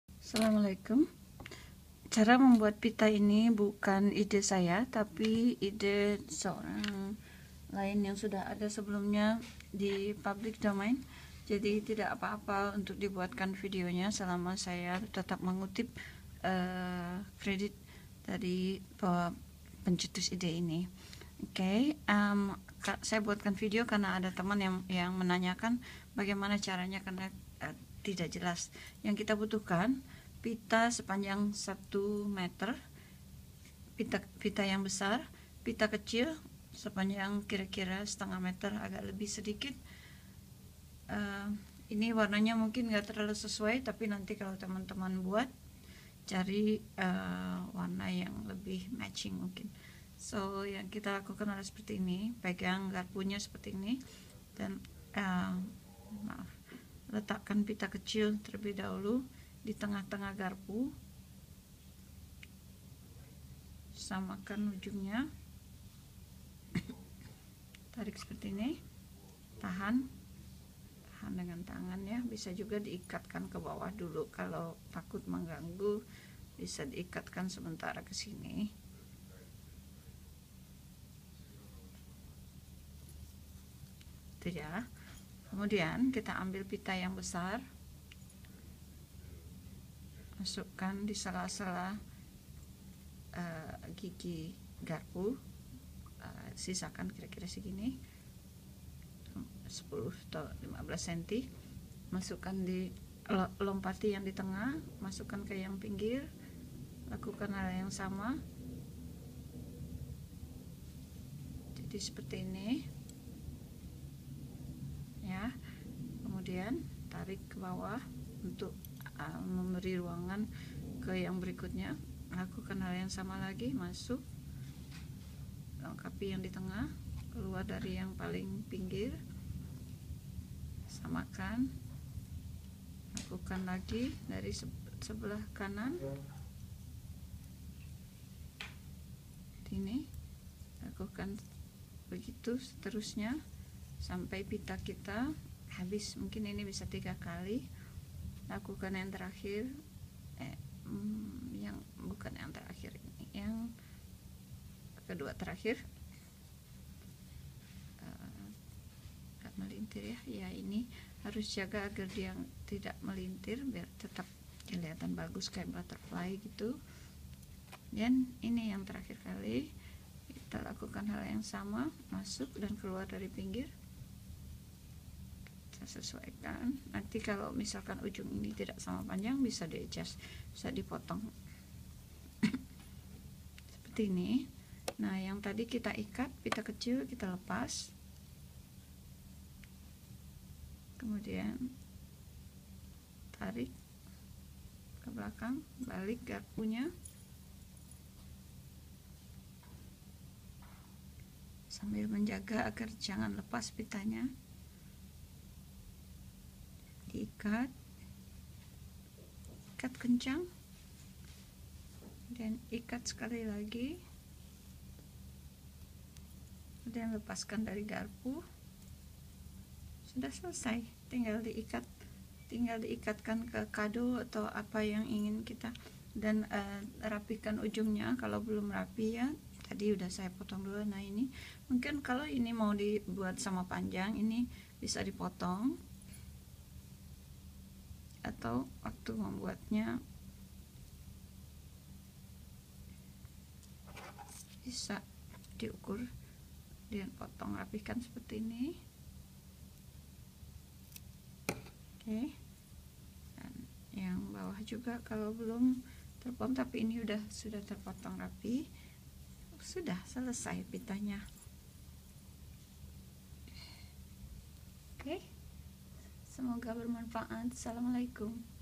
Membuat Hiasan Pita Kupu-Kupu Dengan Garpu
Bahan: Pita lebar 2 cm 1 m, Pita 6 mm 3/4 m, garpu